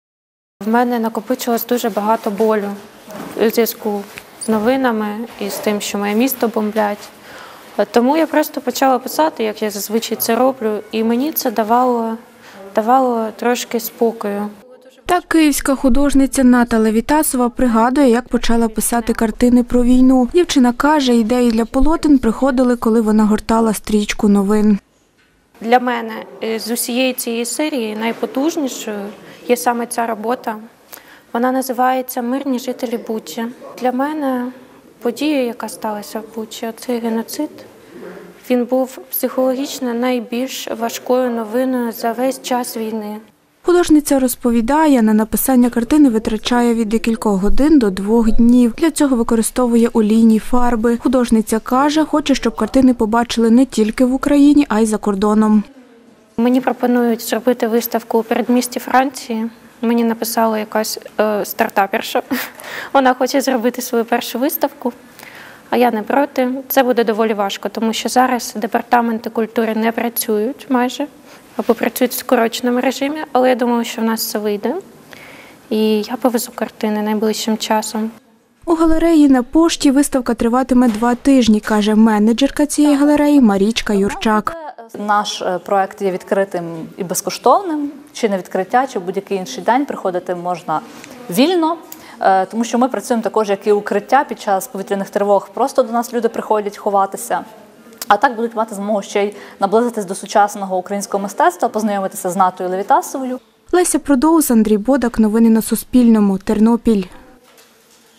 16 картин про війну привезла до Тернополя київська художниця Ната Левітасова
Допоки Україна не переможе, малюватиме лише про війну – київська художниця Ната Левітасова презентувала в Тернополі свої картини. Каже, цю серію створила після того, як російські окупанти почали обстрілювати Бучу. Там, розповідає, живуть її друзі, зараз їхні будинки зруйновані. Про окупацію міста Ната Левітасова намалювала 16 картин. Показала їх у тернопільській галереї "На пошті".